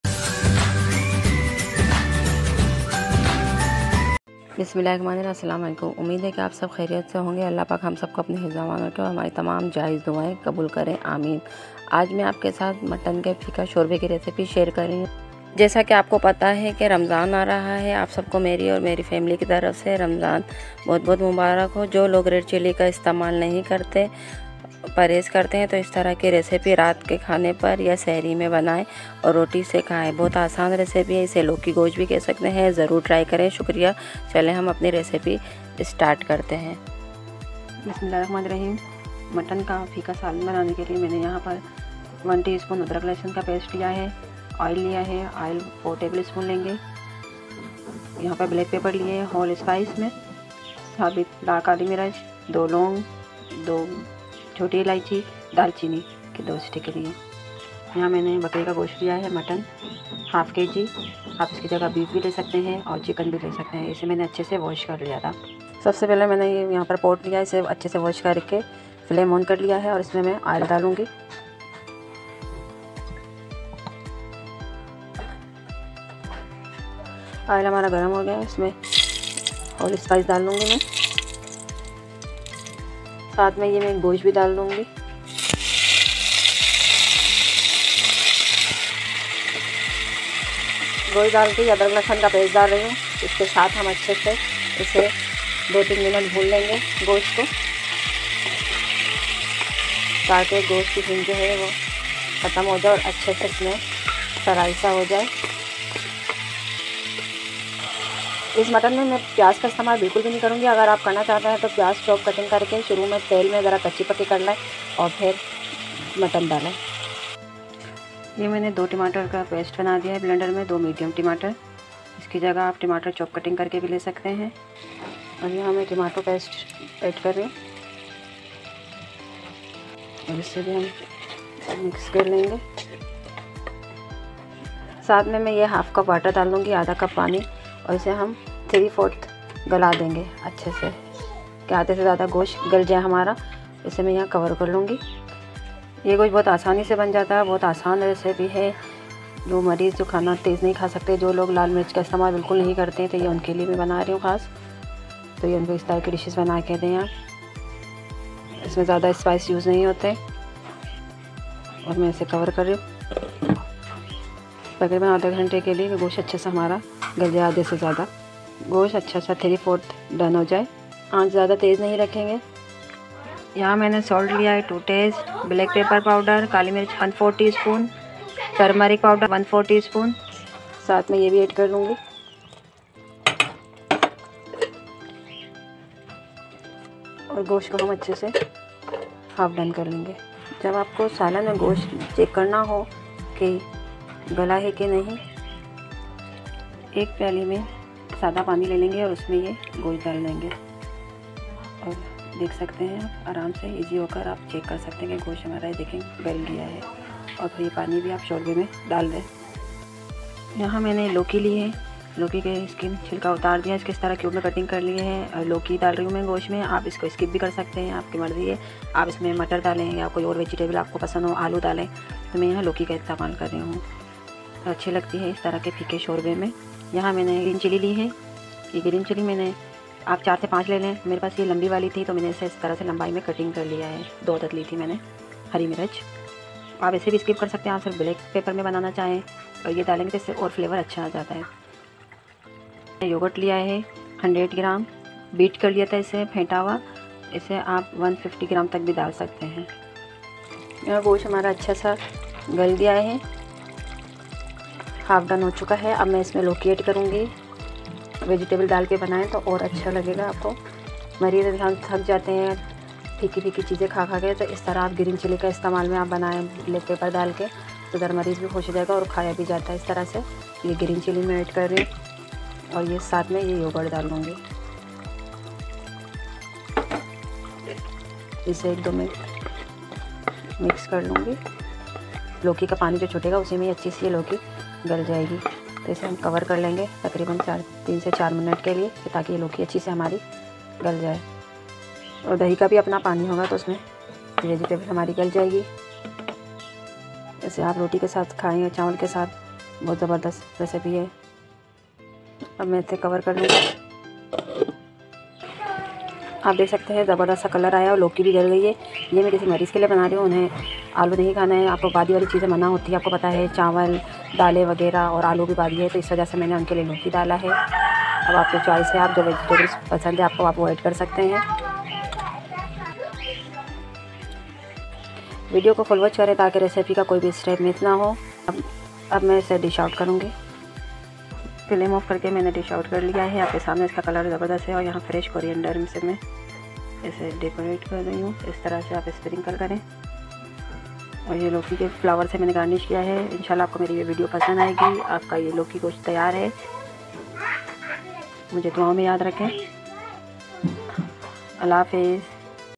Mutton Loki|easy recipe|sehri recipe|Loki gosht|perhez ka salan|ramzan recipe|easy n tasty|dinner
asslam o alaikum AAj ki meri ye Loki mutton ki recipe apko pasand ayegi easy recipe hai Jo log perhez/ red chilli ka istimal nahi krty in k leye hai zarur try kren video ko full watch kren chainl ko like share subscribe comments and support kren jazakallah 💞

shahi custard recipe

kajoor decoration recipe

khajla recipe